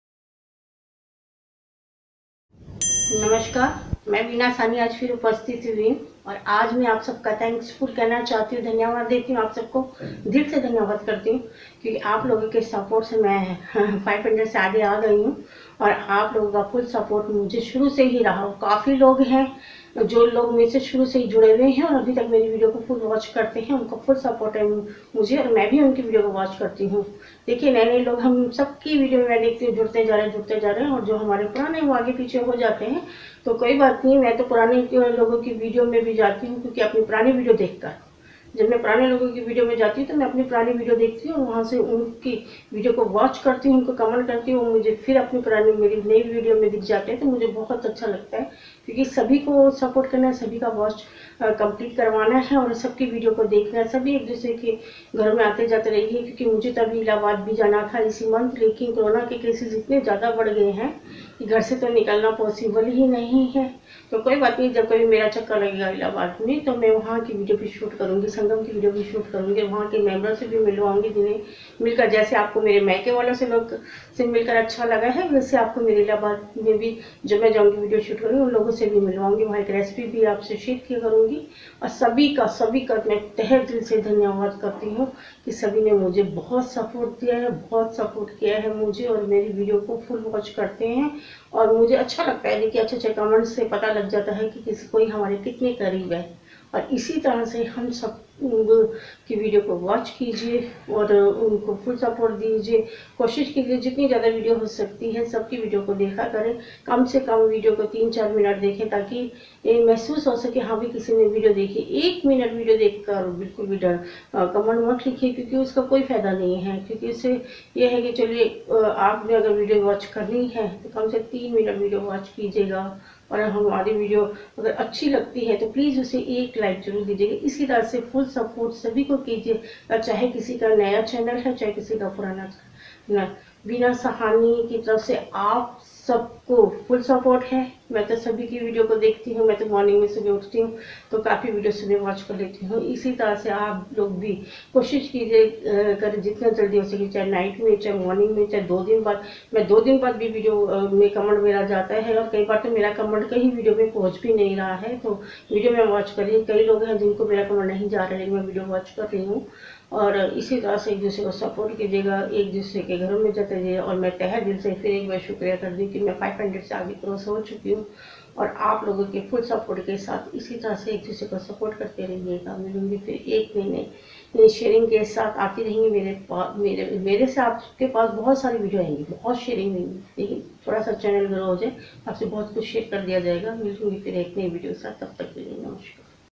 Grateful to achieve 500+ Subscriber | Thank you so much everyone for your support | Beena Sahni
I'm 50 years old mother trying to learn social media and teaching how to cook home made food easily. 👩🏻‍🍳